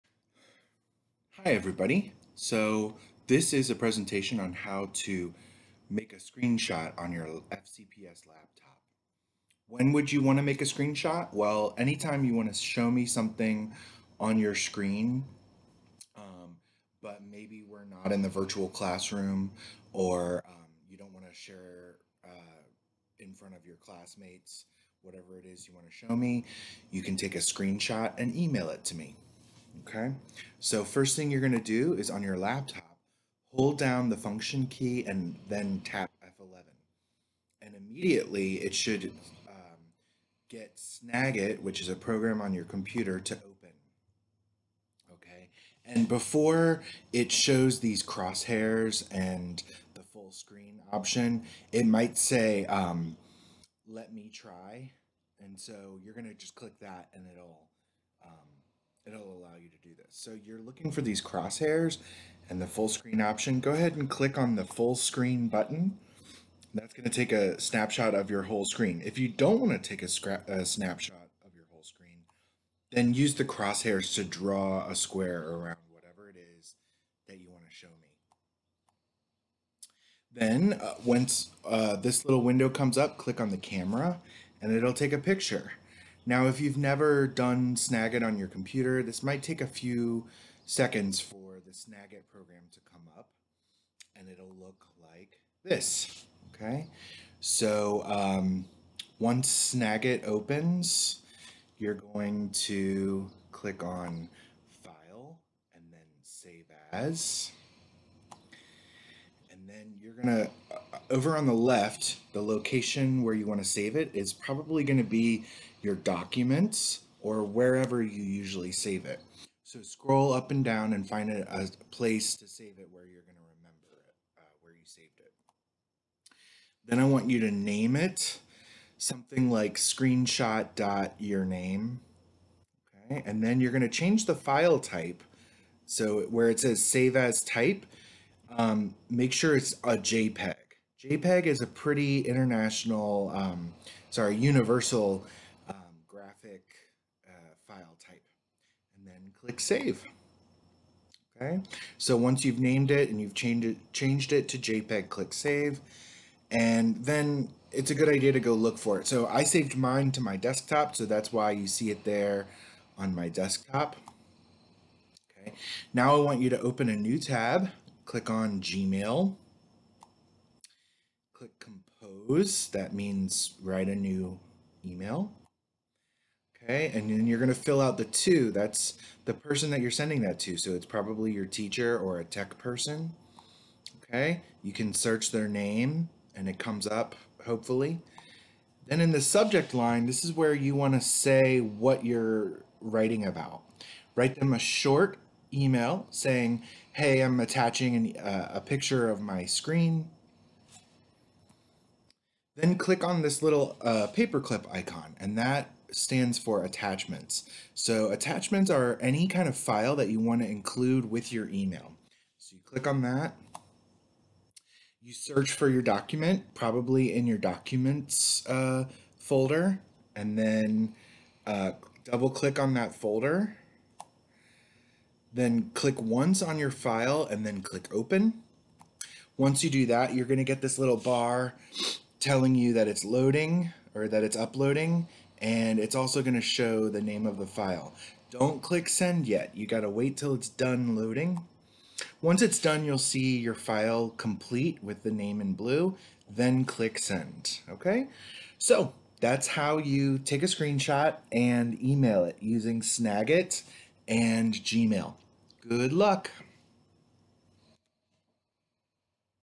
Take a Screenshot and Email It
How to take a screenshot on your FCPS student laptop and email it to someone using Gmail.